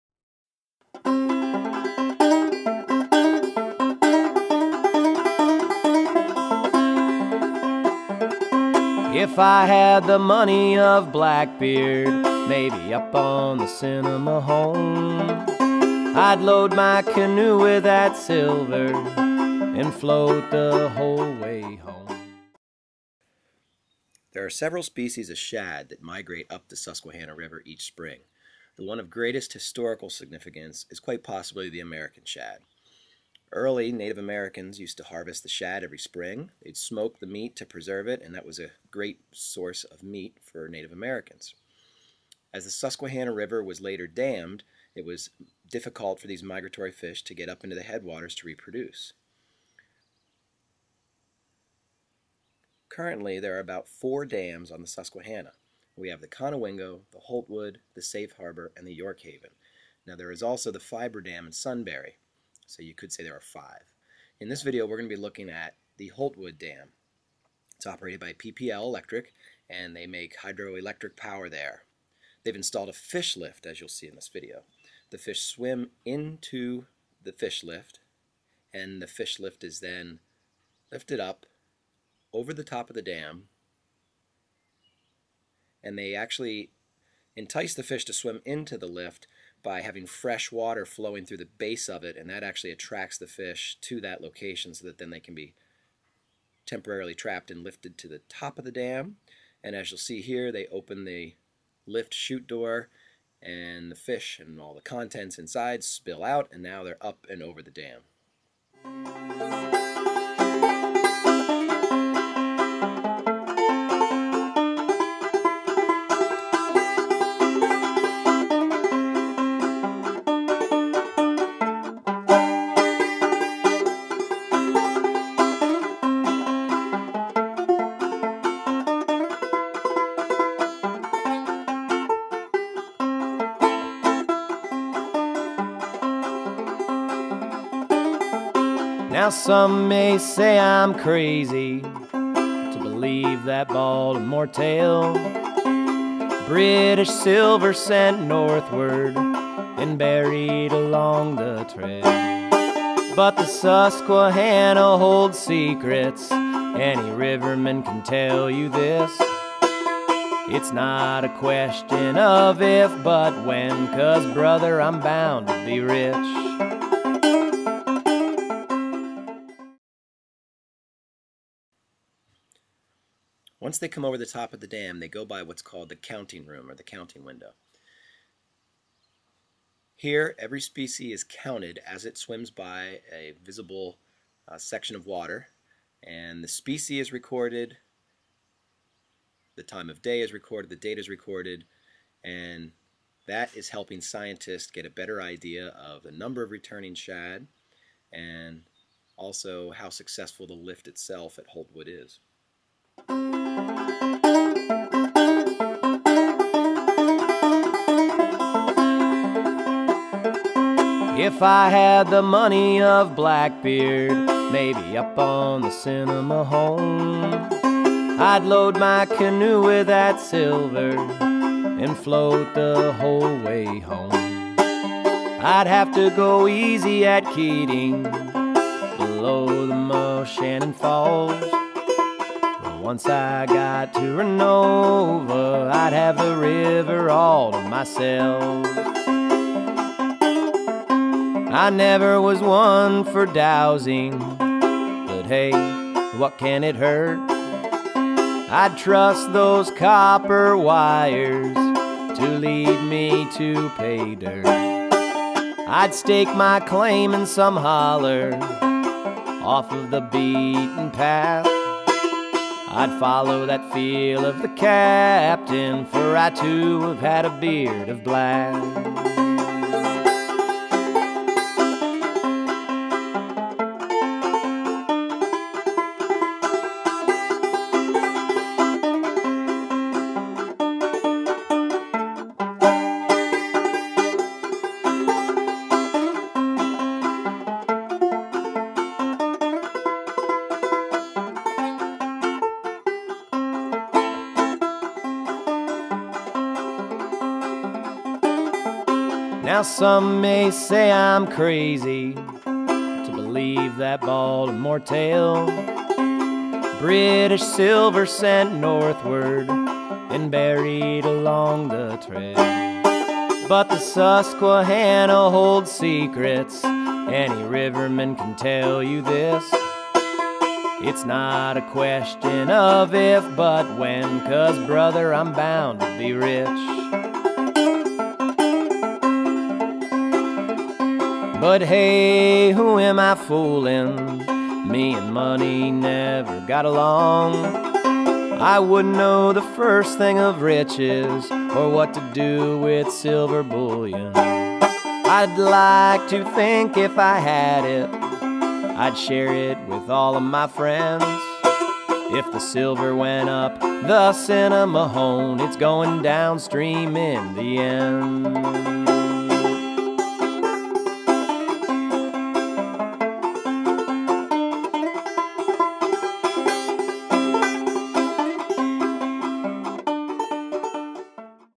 Shad migrating up the Susquehanna River. Holtwood Dam. 2009
Shad are anadromous fish. They swim from the ocean into freshwater streams to spawn in the spring.  This video shows the fishlift at Holtwood Dam lifting Shad over the dam.